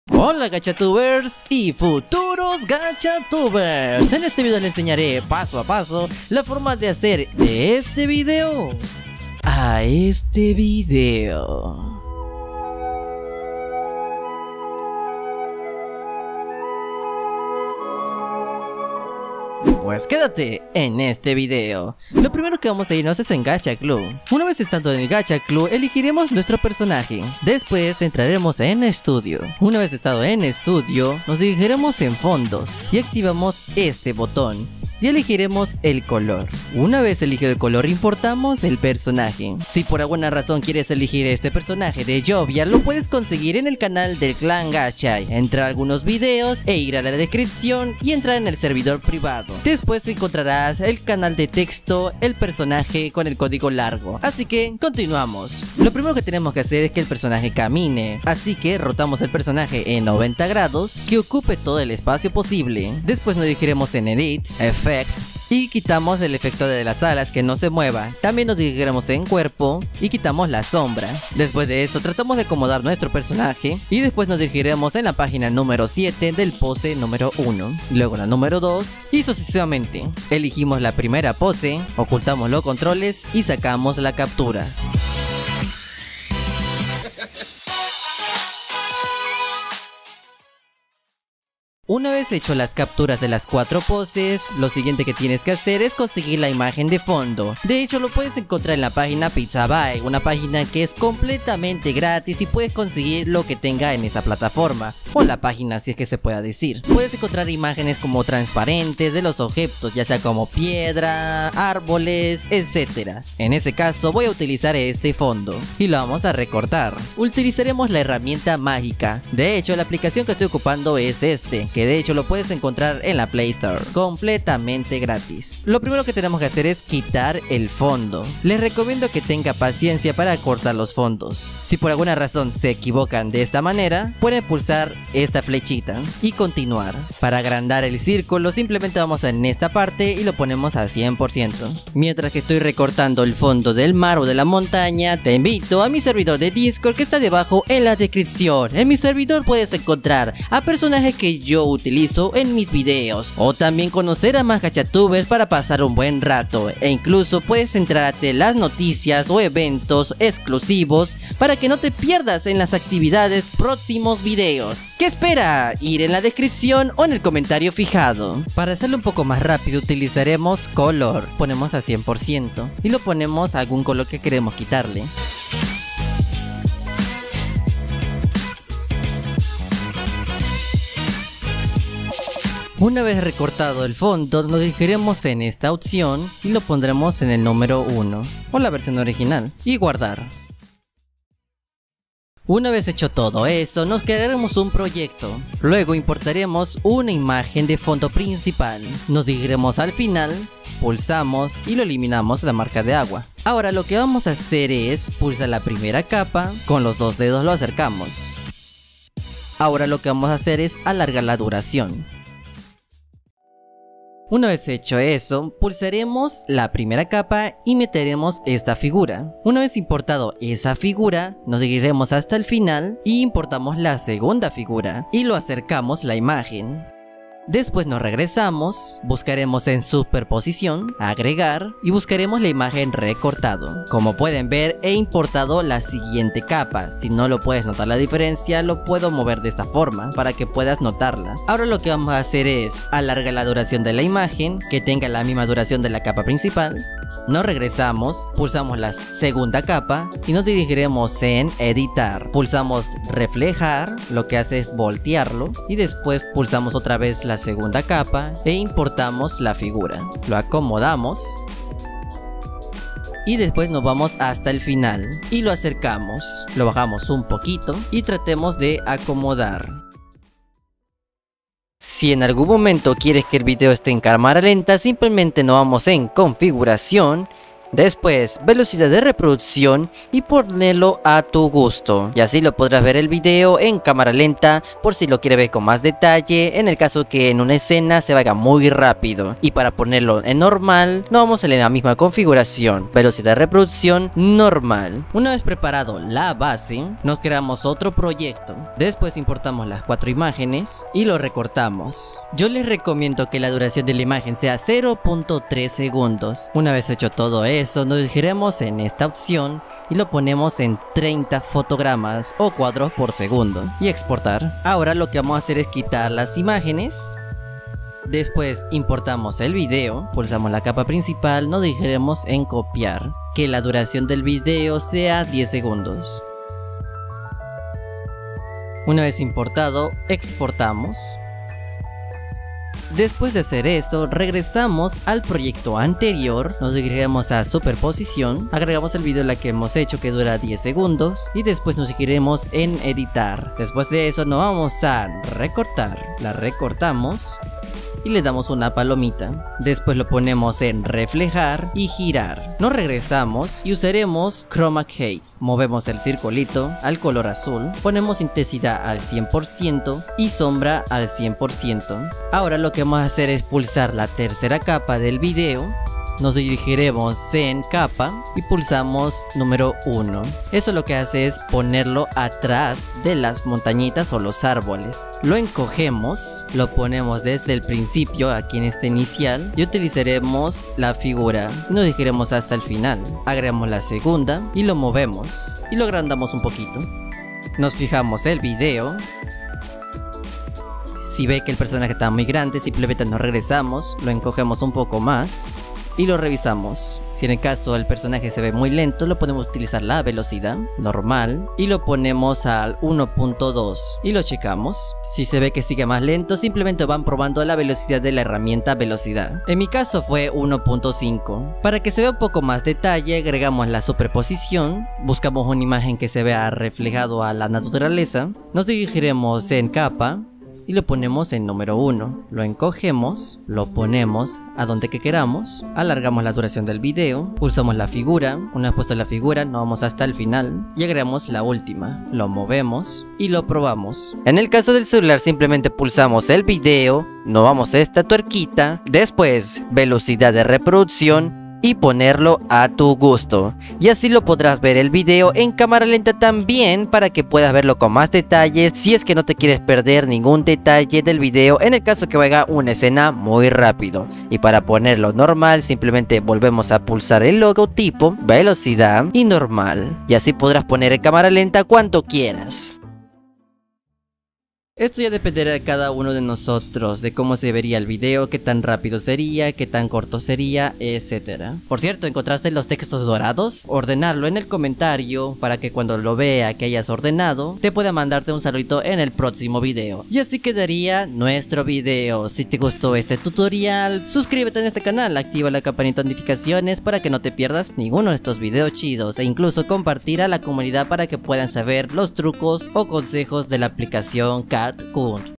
Cómo Hacer Una Cinemática De Gacha Club Con CapCut
3 Aplicaciones recomendadas de editor de video para hacer una animación más fluida sin necesidad de hacer tantas capturas para crear un video, ya sea para series, mini películas de Gacha Club 2024